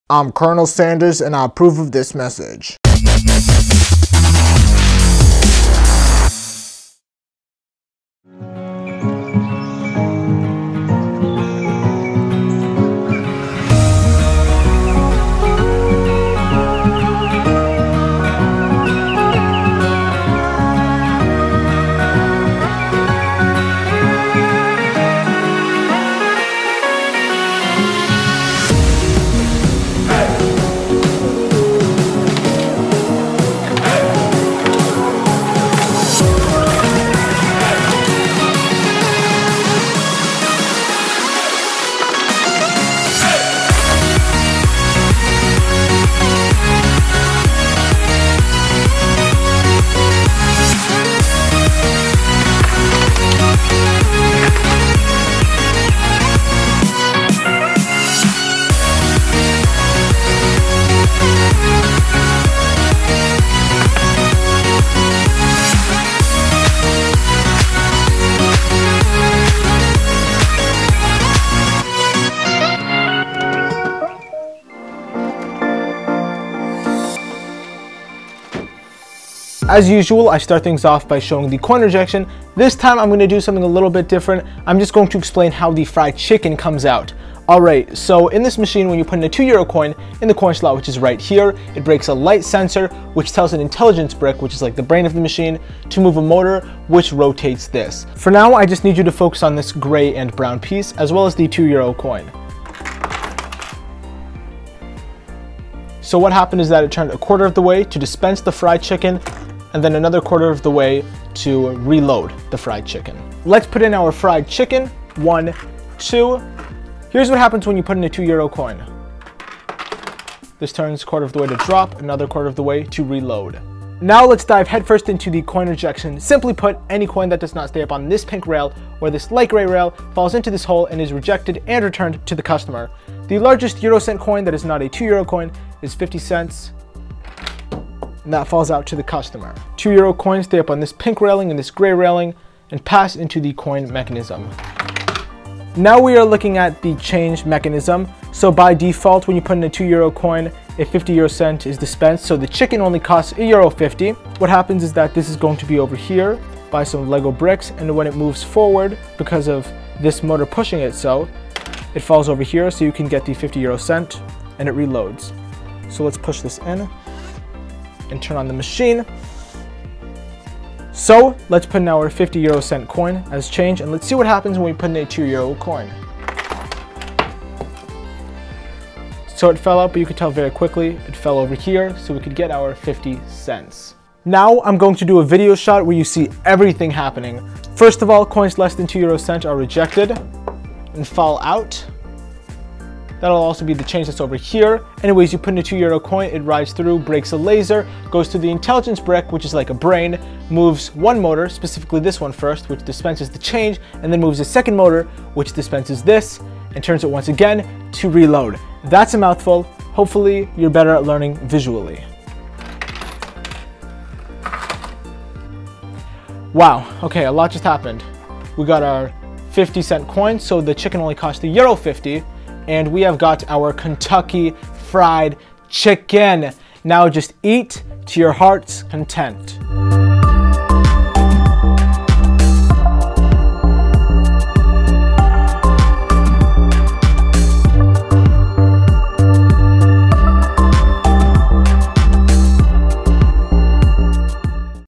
KFC Fried Chicken Custom LEGO Vending Machine
Since a few people suggested the idea, I made today's LEGO machine around KFC.

LEGO® is a trademark of the LEGO Group of Companies, which does not sponsor, authorize or endorse this website.